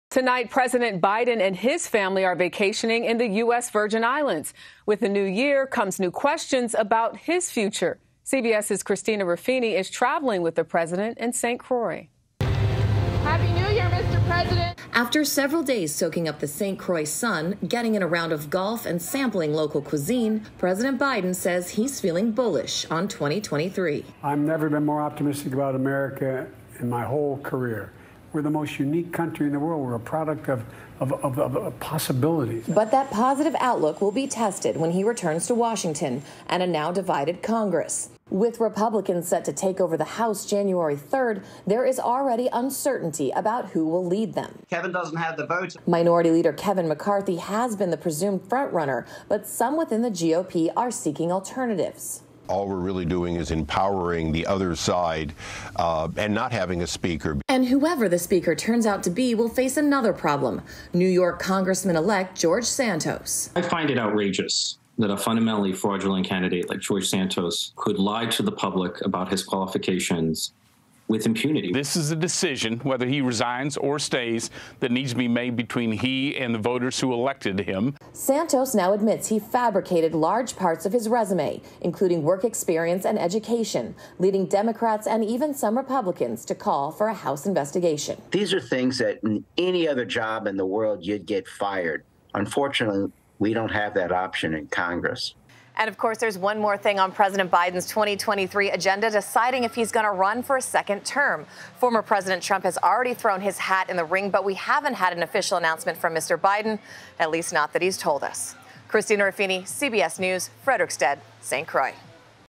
Biden says he is optimistic about America in 2023
According to a recent poll, America's biggest problem is the Government by a large margin
You heard it right, it is neither economy nor poverty nor racism or immigration.
I found this video rather interesting.

Footage: CBS